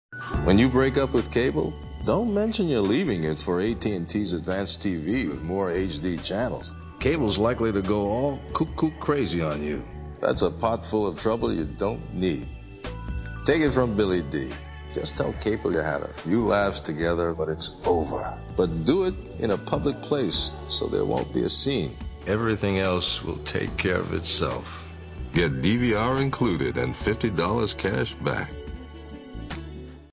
Billy Dee Williams Sells AT&T Advanced TV
Billy Dee Williams, gives some smooth advice, on breaking off your bad cable relationship.